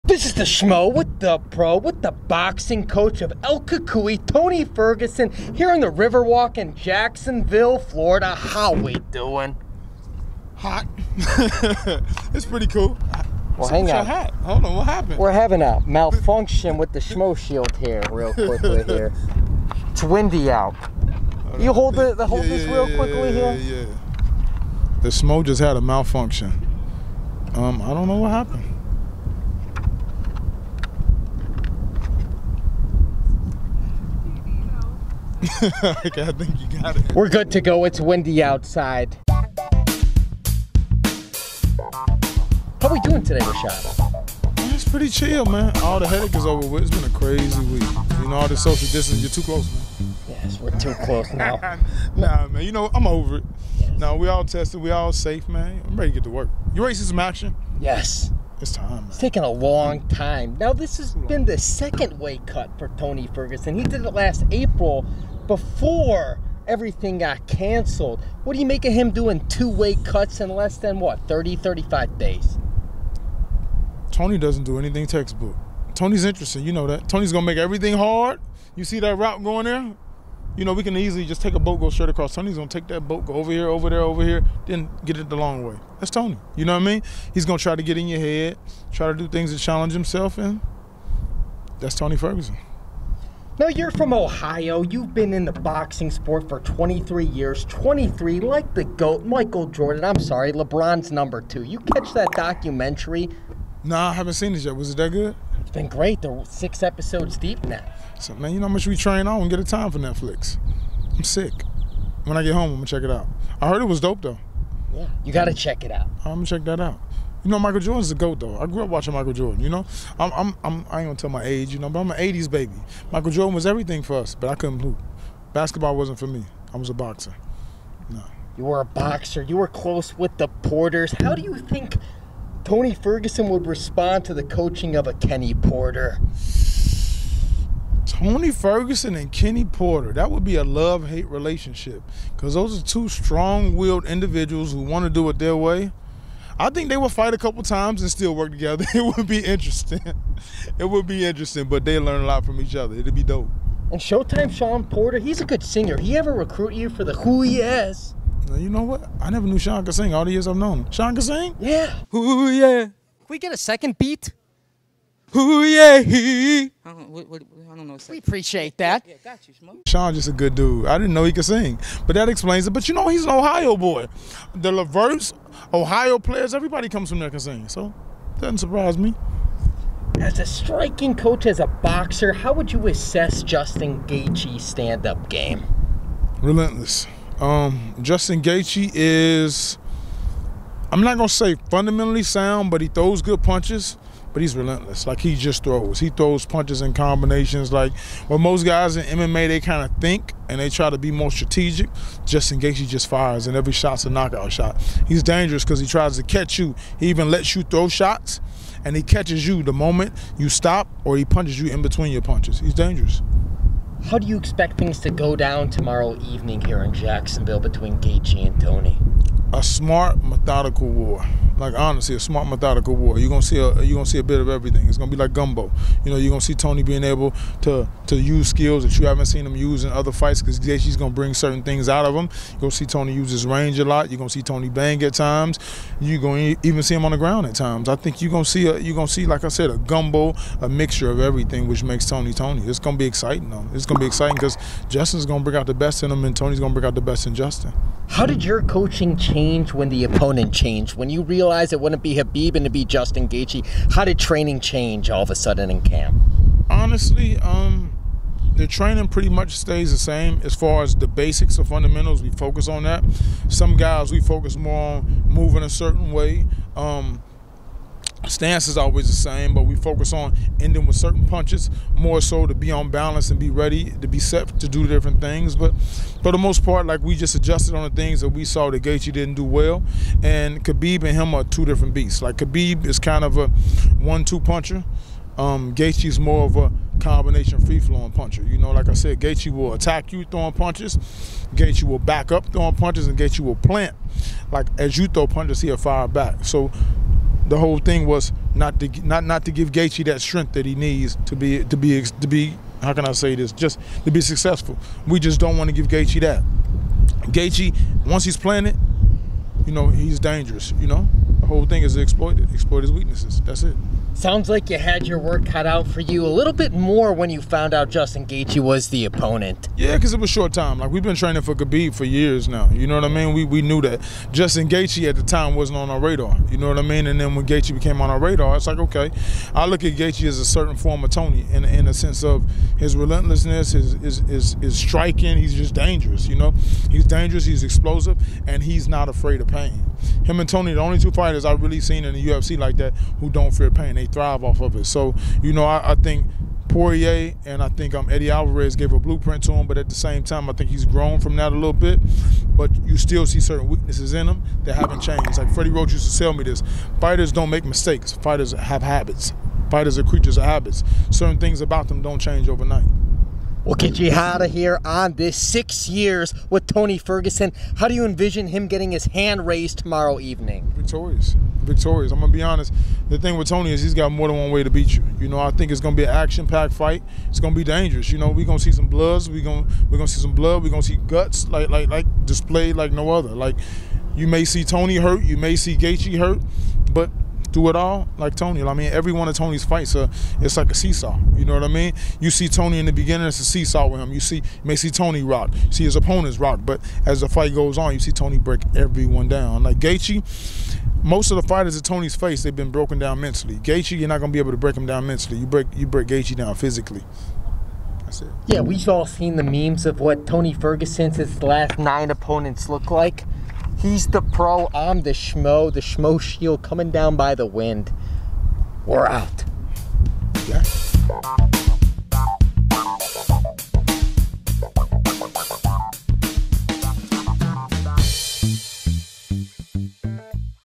Tony Ferguson’s Coach Rashad Holloway Expects Smart Methodical War Against Justin Gaethje
Rashad Holloway is the boxing coach of Tony Ferguson. He joins The Schmo at the Jacksonville Riverfront on the eve of UFC 249 to talk shop. Topics include:

- Two Tony Ferguson Weight Cuts in Less than a month
- Relationship with Former WBC Welterweight Champion Shawn Porter
- Accessing Justin Gaethje’s Standup Game
- Training Differences from Khabib to Justin Gaethje